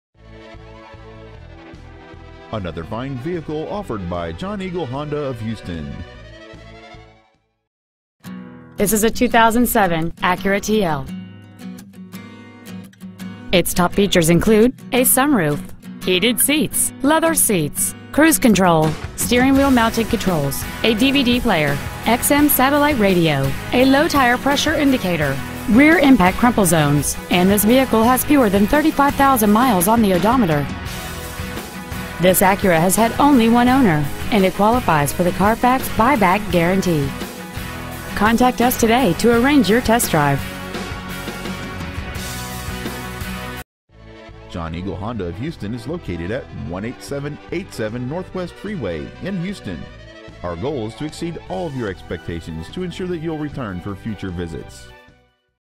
2007 Acura TL Houston TX 77065
Year: 2007
 Make: Acura
 Model: TL
 Engine: 3.2L SOHC PGM-FI 24-valve VTEC V6 engine

 Exterior: Gray
 Miles: 34,616
 Interior: Other

 Preowned 2007 Acura TL Houston TX